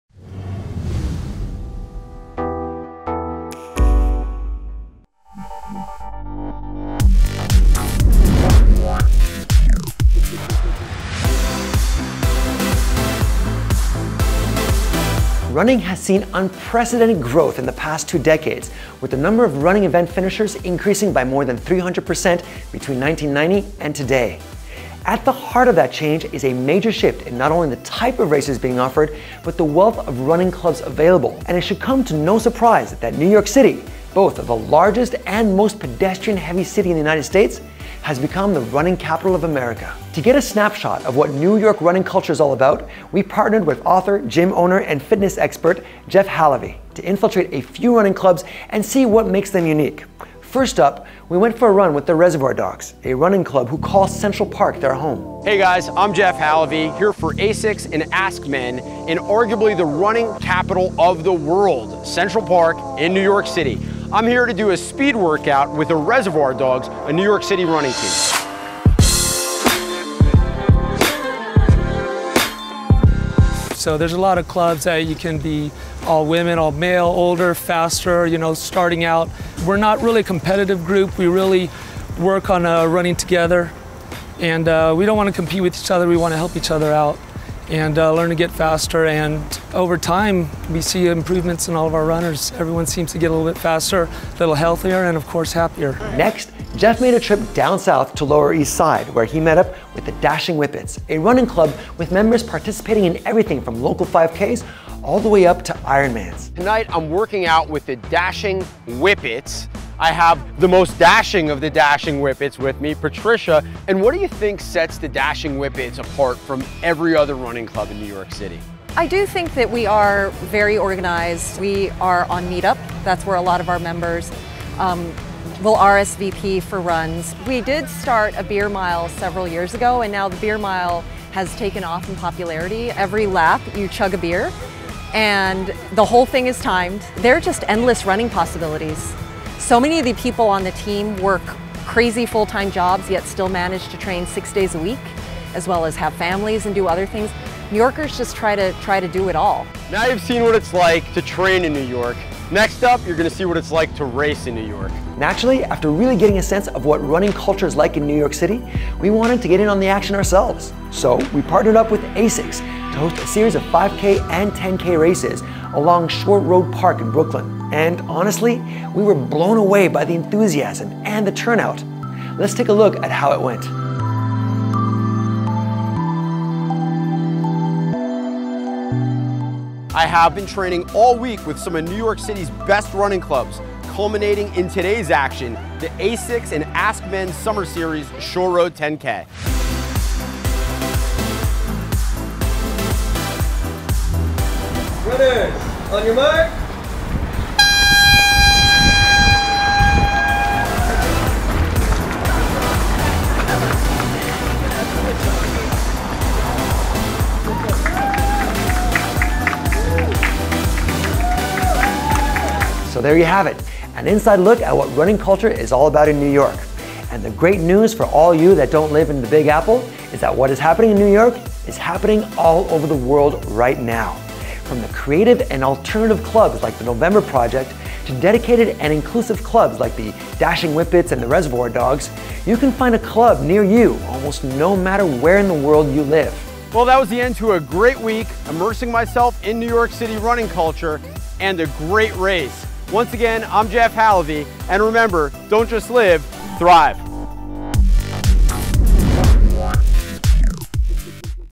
NYC Running Culture | Thrive
Running has seen unprecedented growth in the past two decades, with the number of running event finishers increasing by more than 300% between 1990 and today. At the heart of that change is a major shift in not only the type of races being offered, but the wealth of running clubs available, making it easy for anyone to find a club full of like-minded people at a similar skill level. And it should come as no surprise that New York City, both the largest and most pedestrian-heavy city in the united states, has become the running capital of America.

To get a snapshot of what New York running culture is all about, we partnered with author, gym owner, and fitness expert Jeff Halevy to infiltrate a few running clubs and see what make them unique. First up, he went for a run with the Reservoir Dogs, a running club who call Central Park their home.